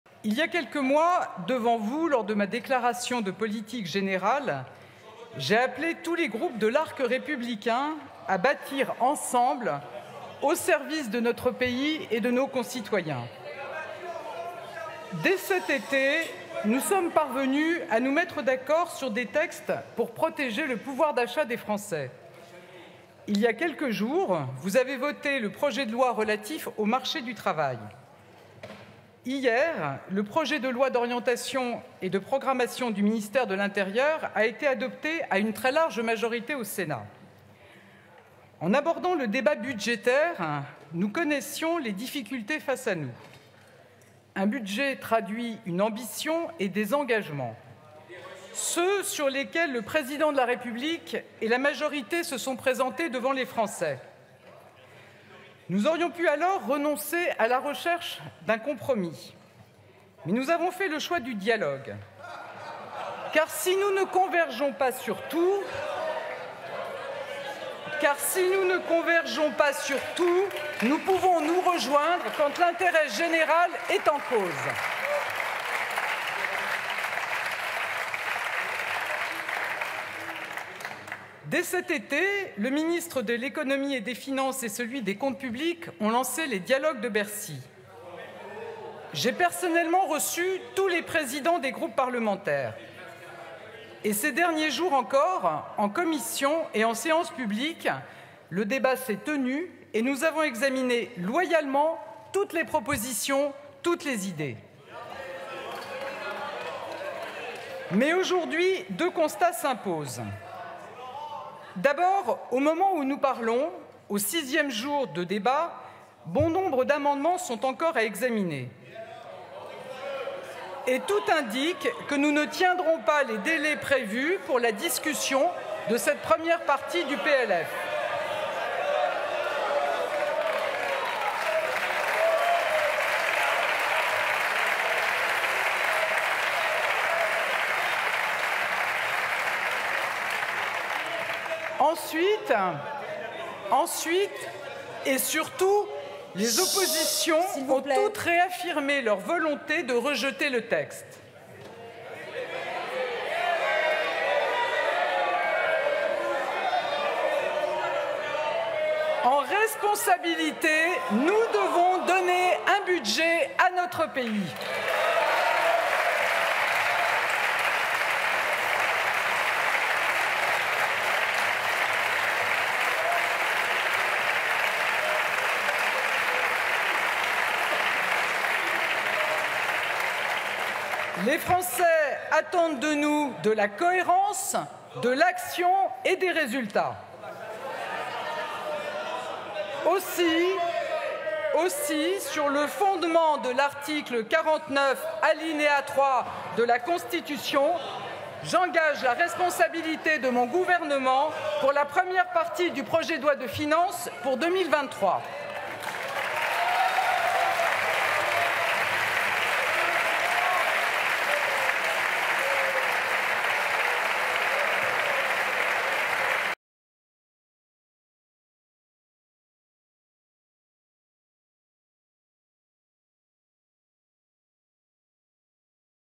Budget 2023 : Elisabeth Borne dégaine le 49-3
La Première ministre a annoncé devant les députés mettre en jeu la responsabilité du gouvernement sur le Budget. La gauche a immédiatement annoncé le dépôt d'une motion de censure, mais elle est vouée à l'échec, et le texte sera ainsi adopté sans vote.